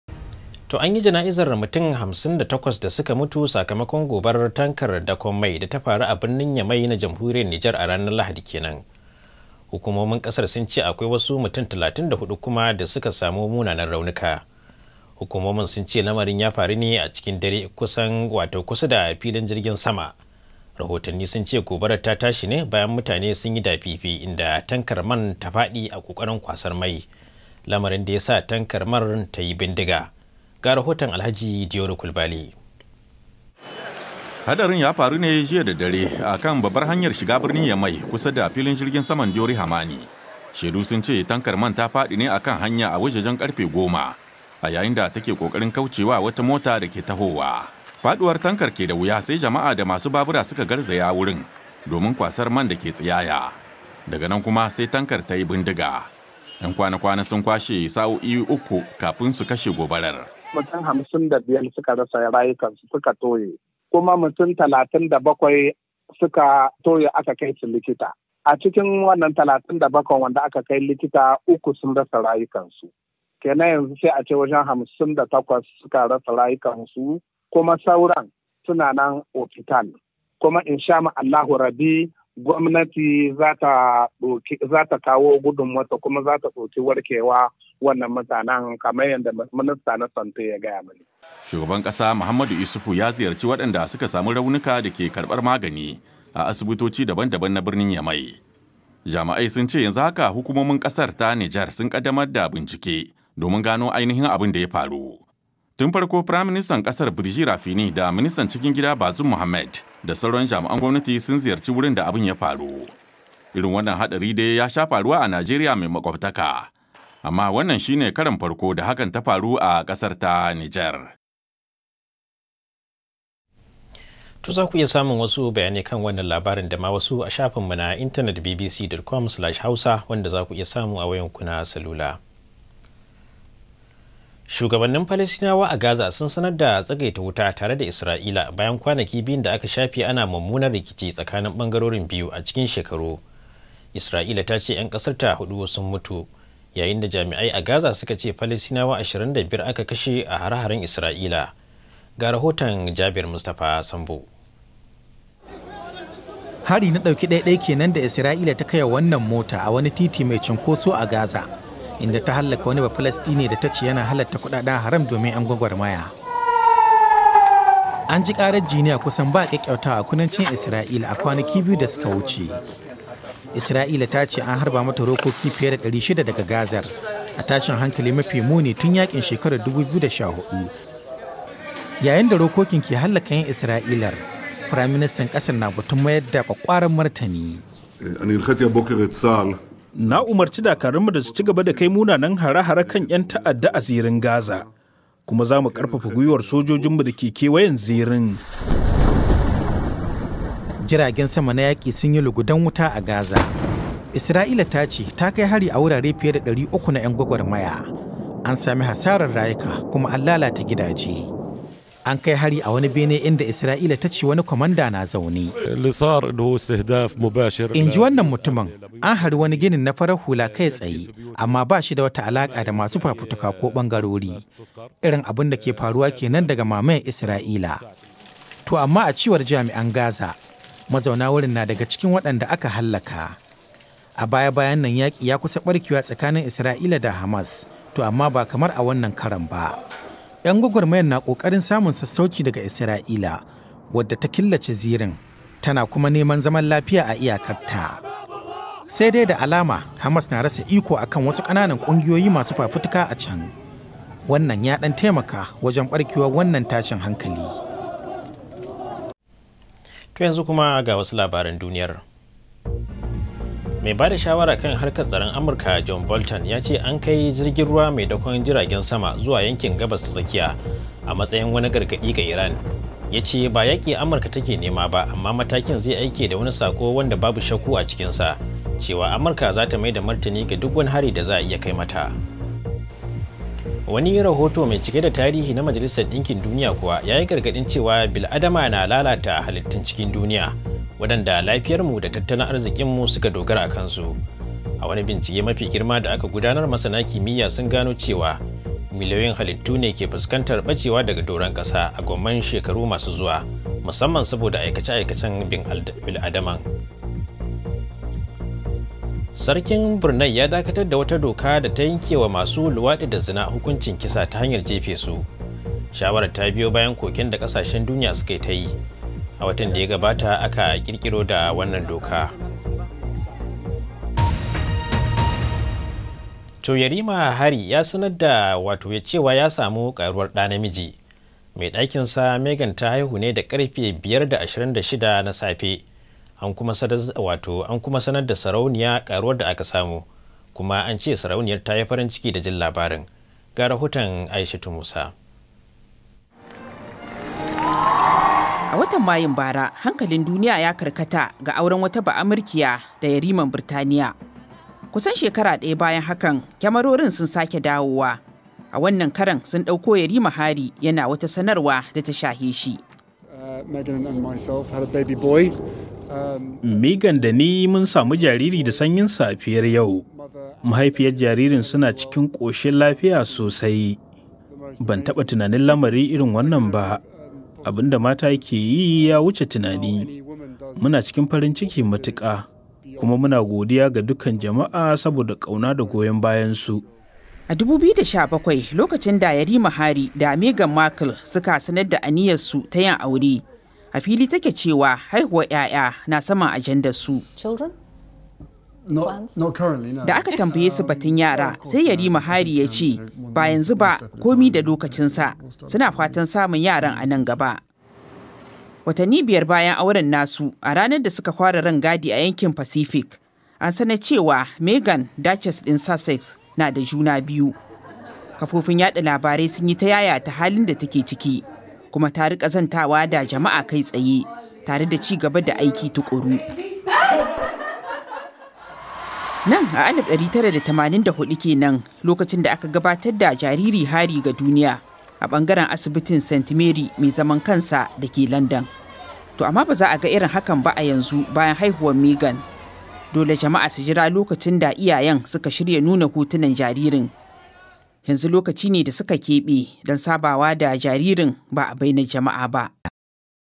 An yi jana'izar mutanen da suka mutu a gobara a Nijar
***An yi jana'izar mutum 58 da suka mutu, sakamakon gobarar tankar mai da ta faru a birnin Yamai na Jamhuriyar Nijar a ranar Lahadi.
***Shugabannin Palasdinawa sun ce an cimma yarjejeniyar tsagaita wuta tare da Israila, bayan kwanaki biyun da aka kwashe ana rikici.
***An haifi jariri namiji a gidan Sarautar Burtaniya, wanda shi ne na bakwai a jerin mutanen da ka iya gadon sarautar.